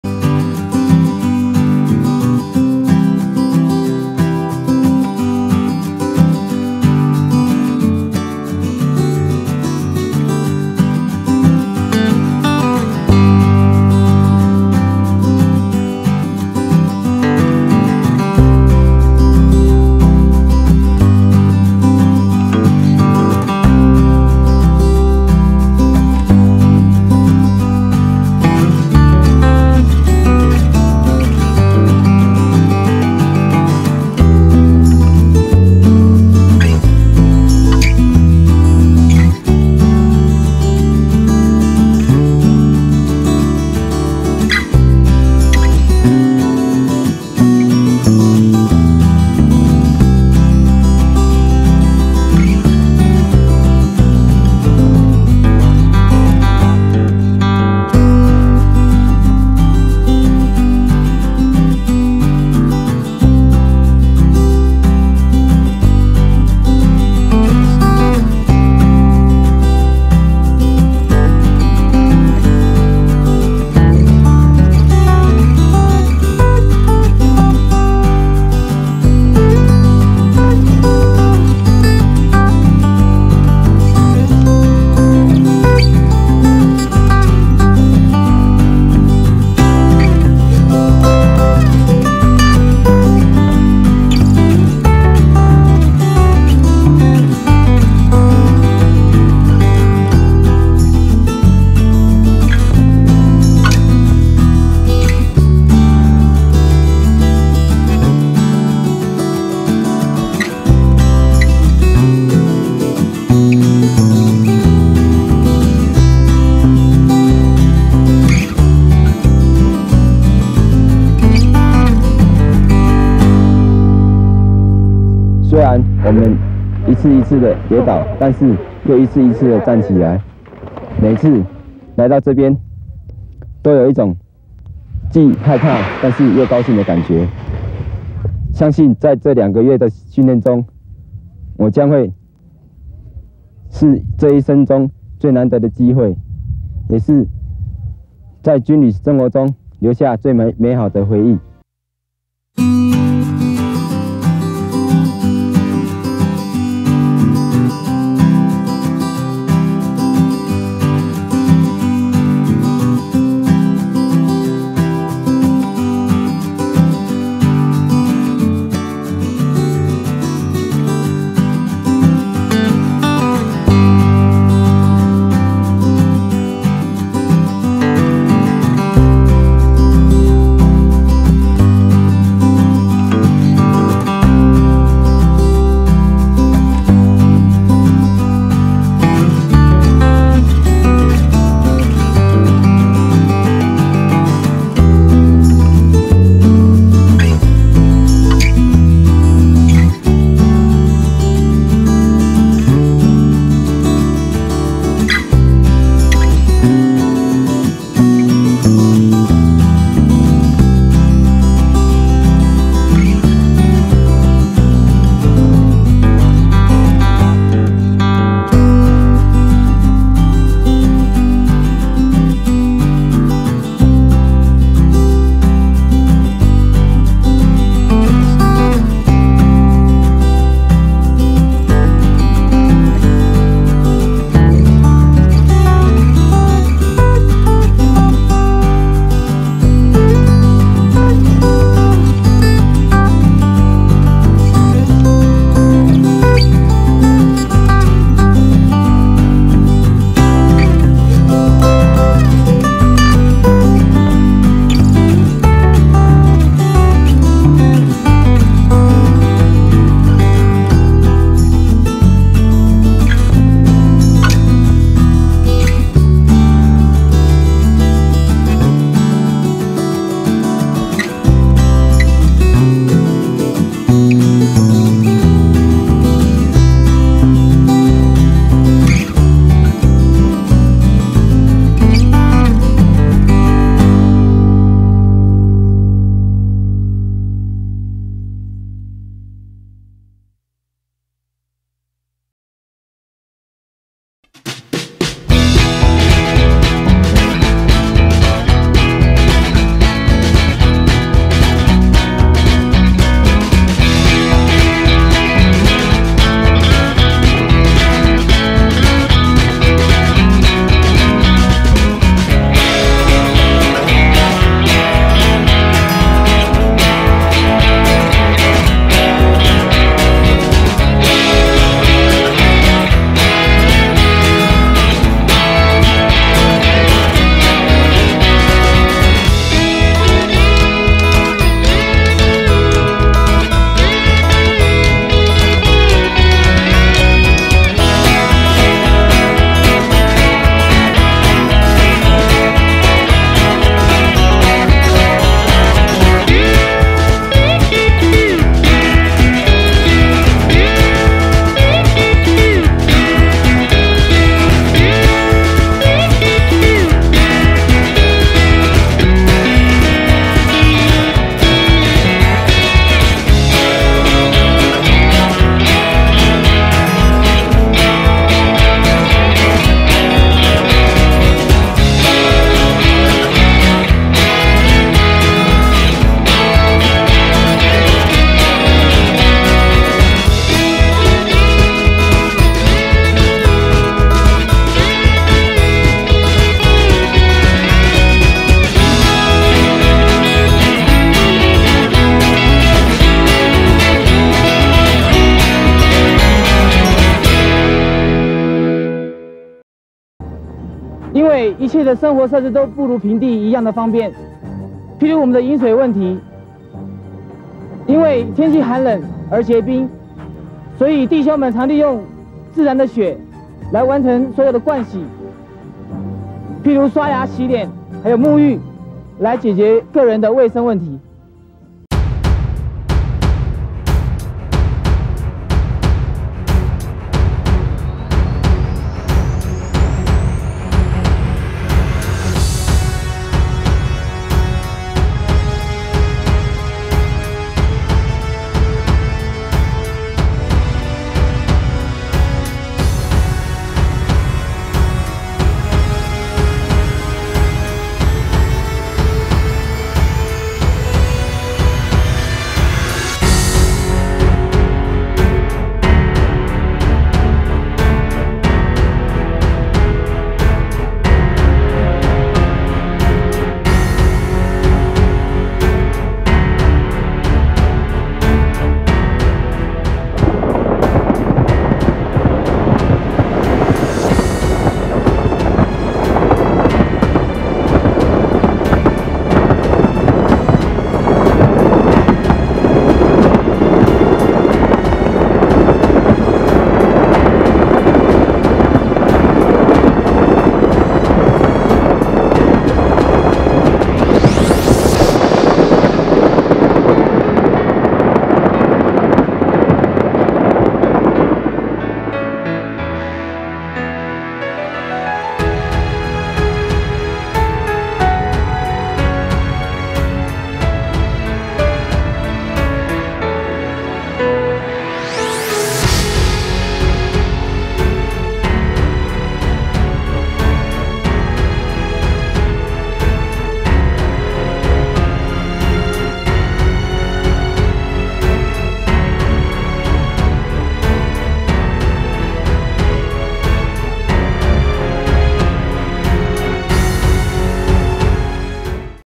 民國八十年代國軍合歡山武嶺寒訓實況
當兵還可以學滑雪羨慕吧！
國軍在合歡山武嶺的寒訓基地曾經調訓部隊進行山、寒地作戰訓練。
影片中連隊受訓期間正逢瑞雪，除了滑雪、堆雪人、打雪仗外，還學會做冰屋，為當兵的歲月留下美好的回憶！

1992年春海軍運補南沙太平島陸戰隊勇士(一)

1992年春海軍運補南沙太平島陸戰隊勇士(二)

1992年春戍守南沙太平島國軍戰備實況